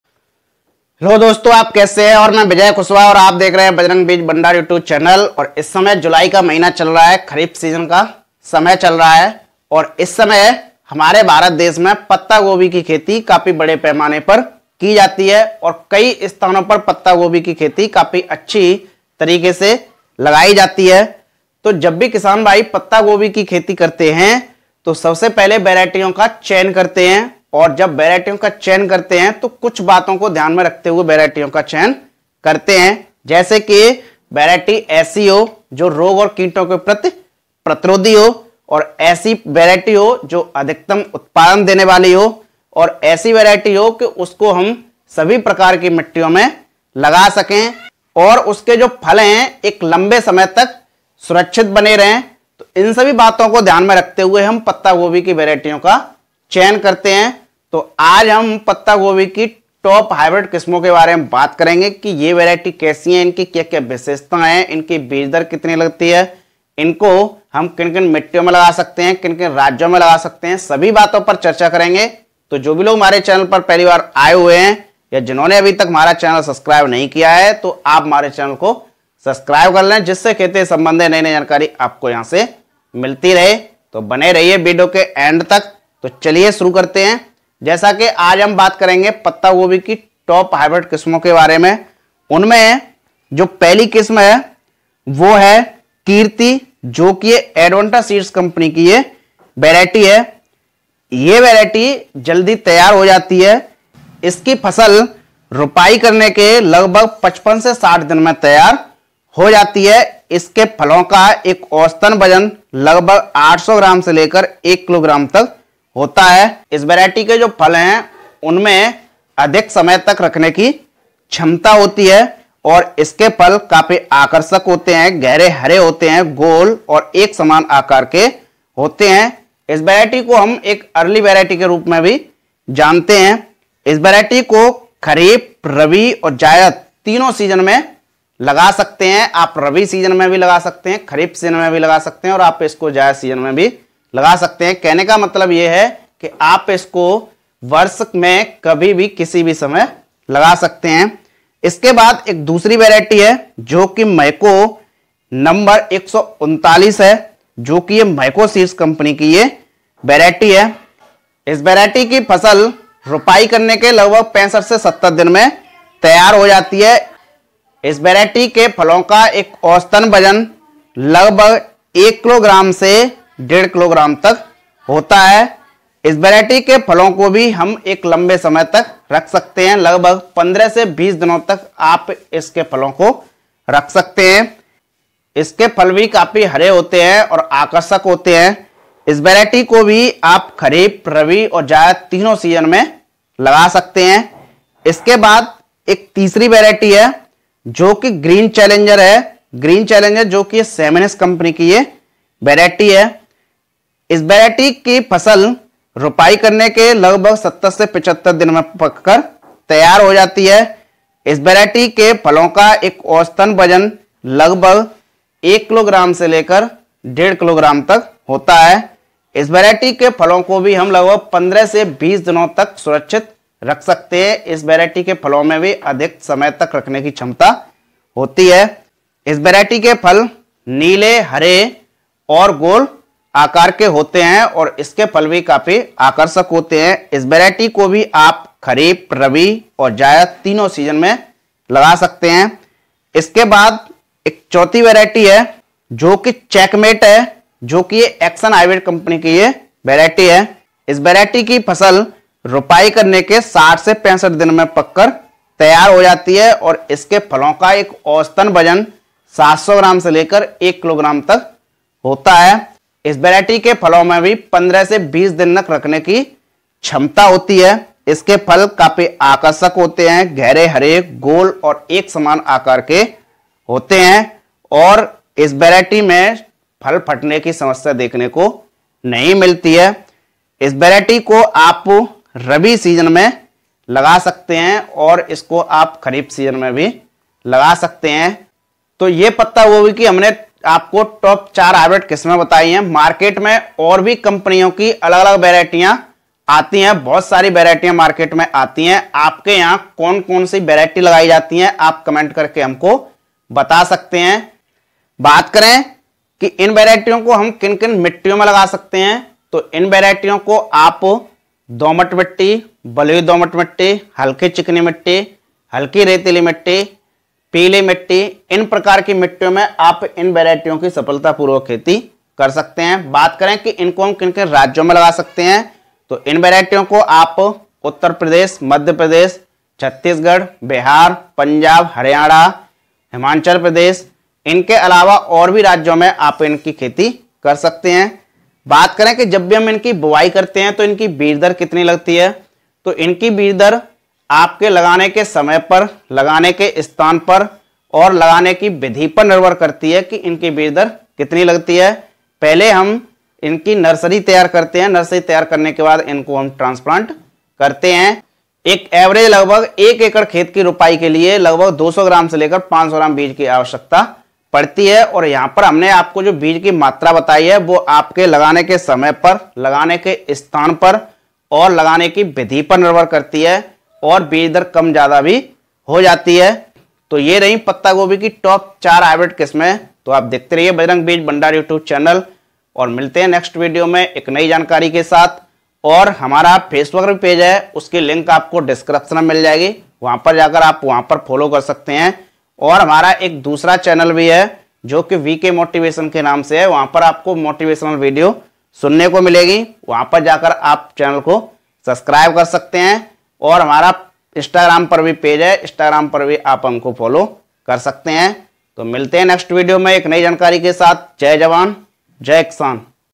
किसानों की गरीबी मिटाने वाली पत्तागोभी की टॉप 4 हाइब्रिड किस्में।। Cabbage top 4 hybrid varieties।।
किसानों की गरीबी मिटाने वाली बंदगोभी की टॉप 4 हाइब्रिड किस्में।। Cabbage top 4 hybrid varieties।।